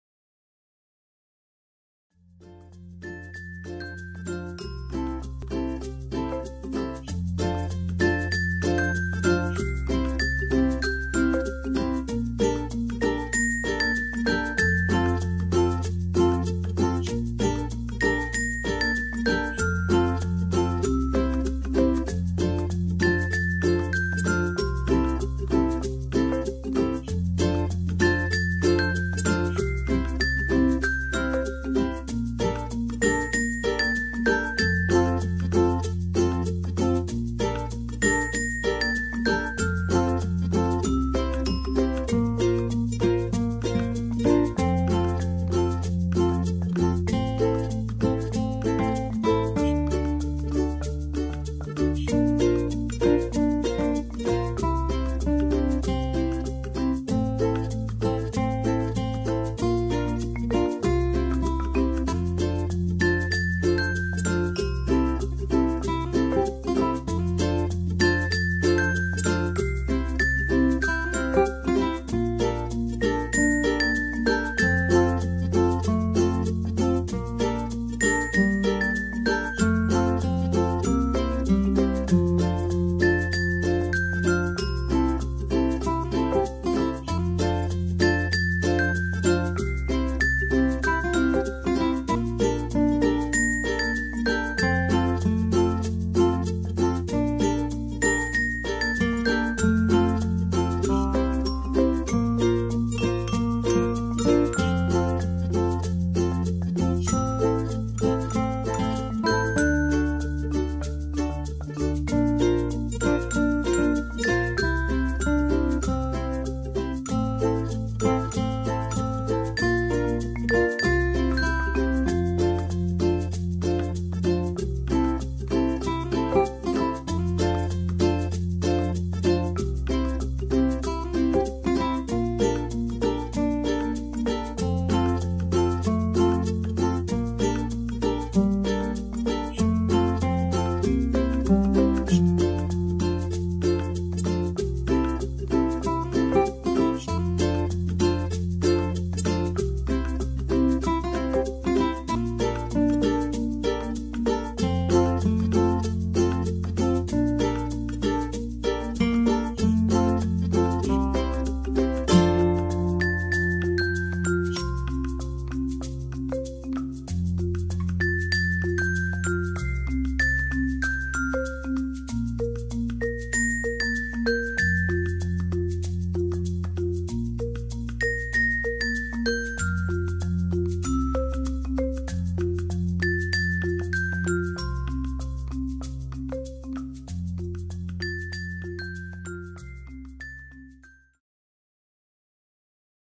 3 Bedroom Townhouse for sale in Eastern Cape | Port Elizabeth | Lorraine
3 Bedroom Townhouse in Lorraine, Superb new freehold estate in Lorraine - very secure - elevated views. Available homes - 2 complete